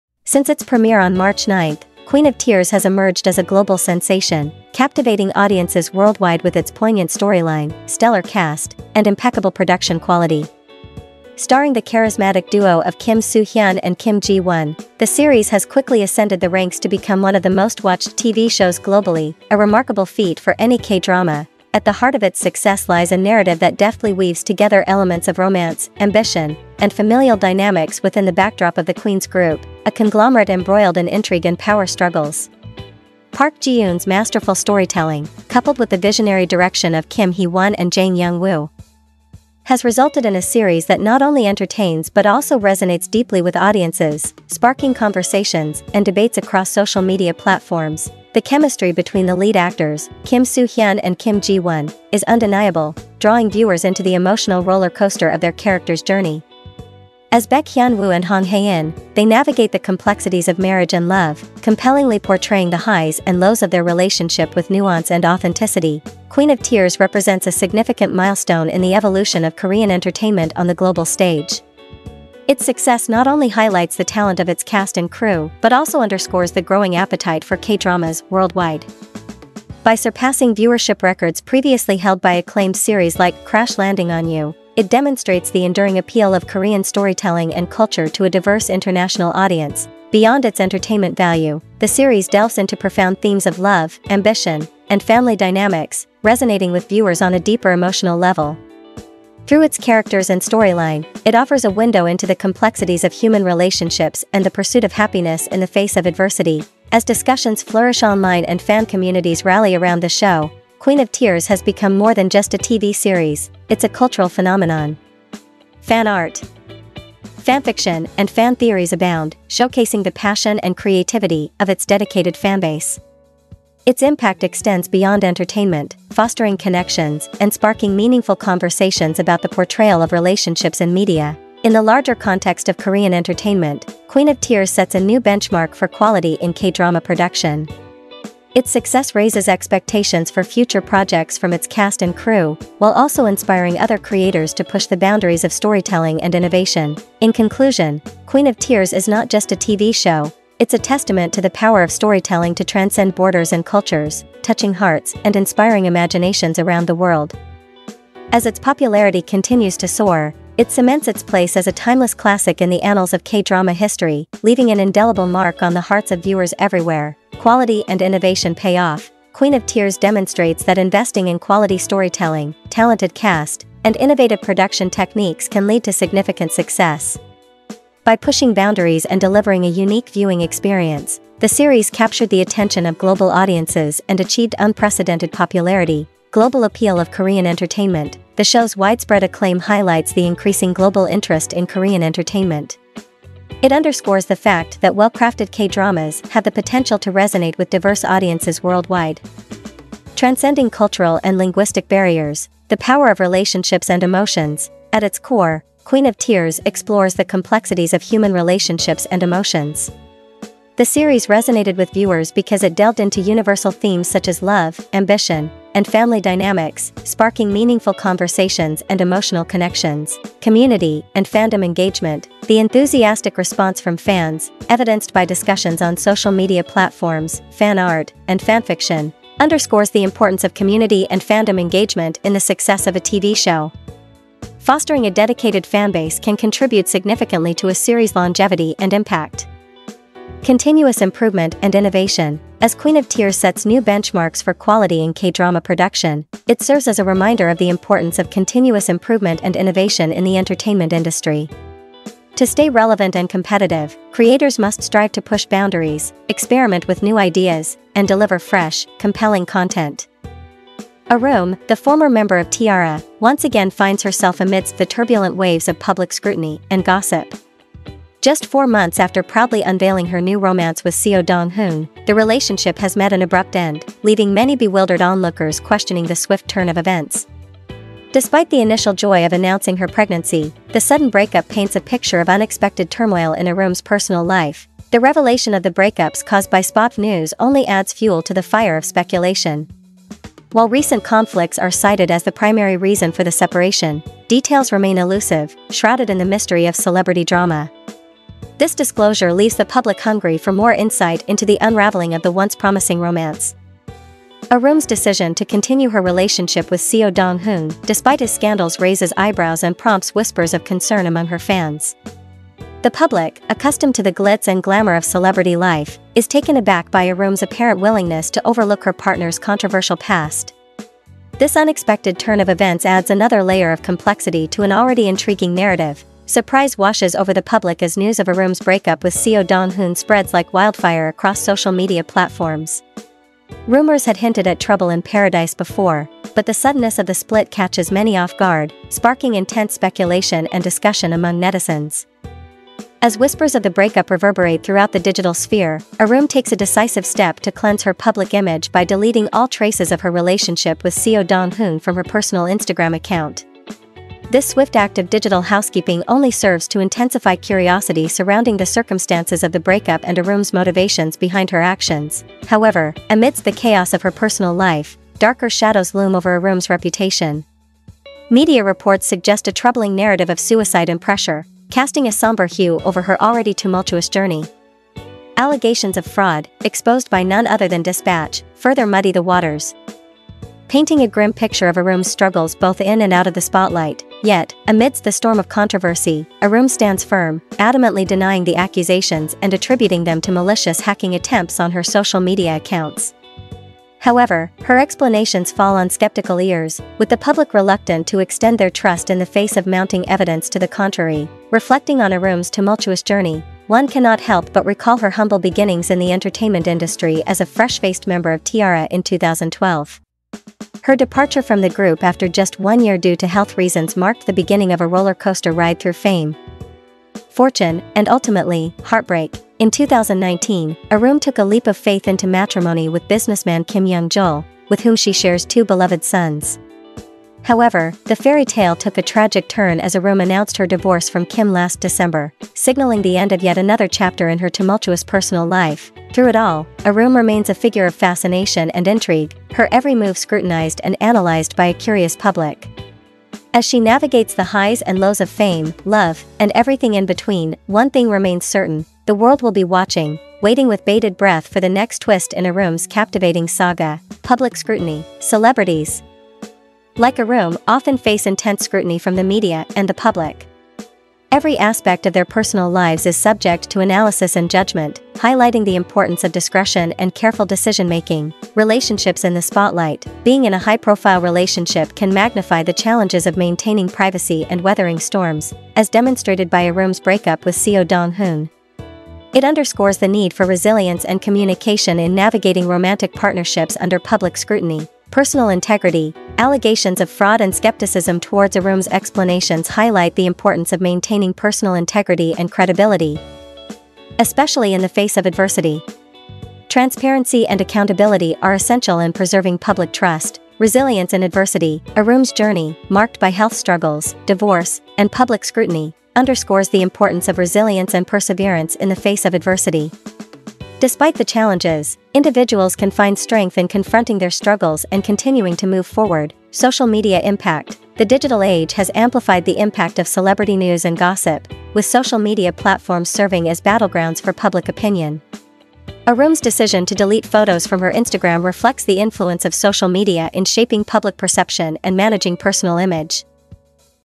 'Queen of Tears' with Kim Soo Hyun, Kim Ji Won Takes Global Audiences by Storm, Ranks 9th in Viewing
ACNFM - Asian Celebrities News: At the heart of its success lies a narrative that deftly weaves together elements of romance, ambition, and familial dynamics within the backdrop of the Queens Group, a conglomerate embroiled in intrigue and power struggles. Park Ji Eun's masterful storytelling, coupled with the visionary direction of Kim Hee Won and Jang Young Woo, has resulted in a series that not only entertains but also resonates deeply with audiences, sparking conversations and debates across social media platforms.